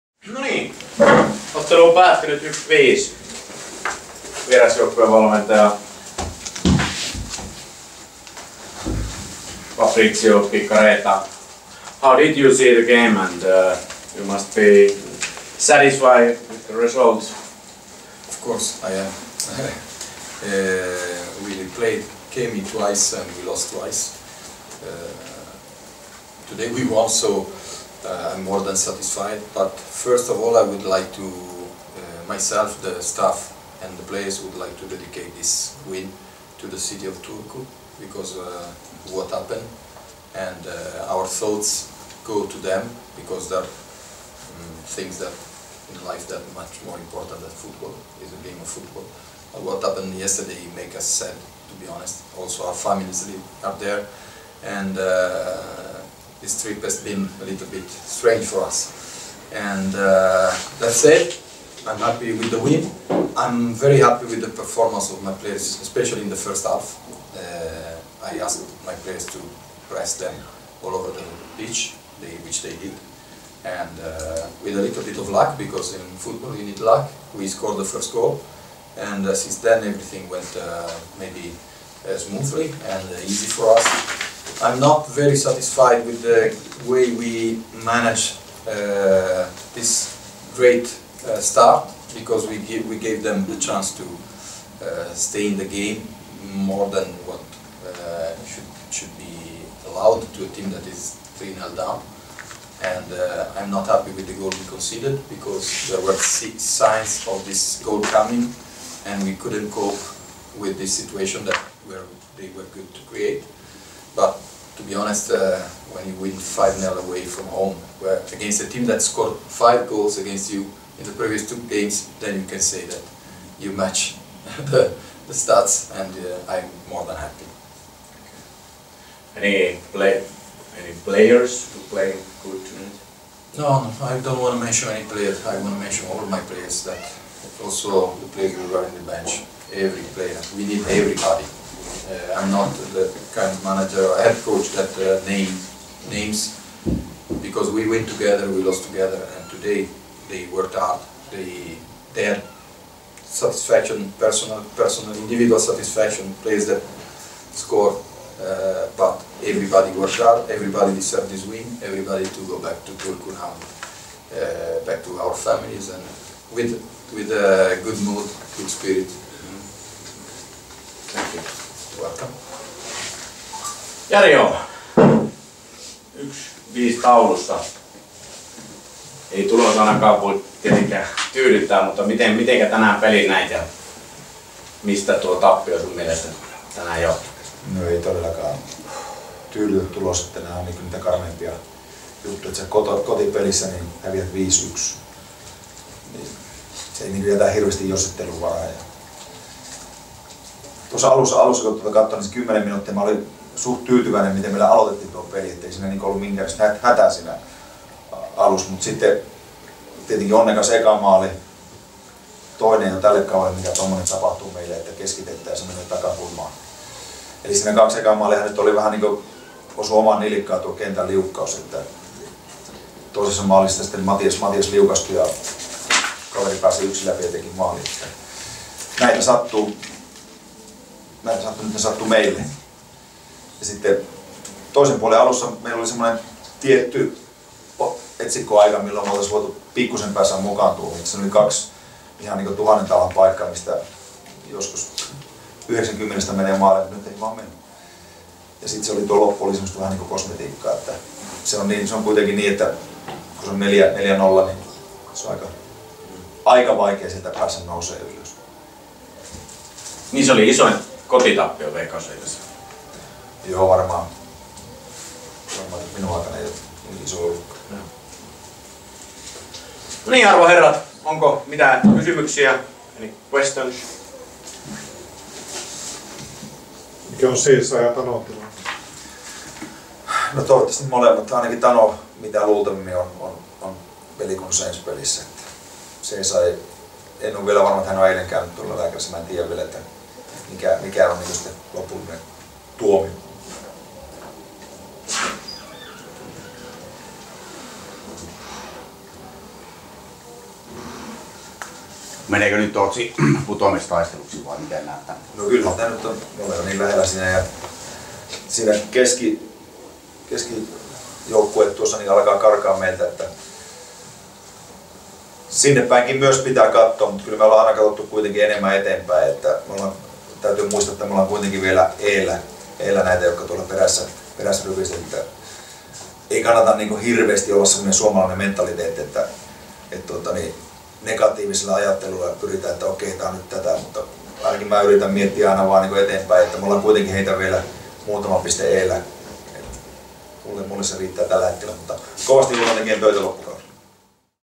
Lehdistötilaisuus PS Kemi - FC Inter 19.08.17
Paikalla Jari Åhman ja Fabrizio Piccareta, joka omisti voiton tragedian kohdanneelle Turulle.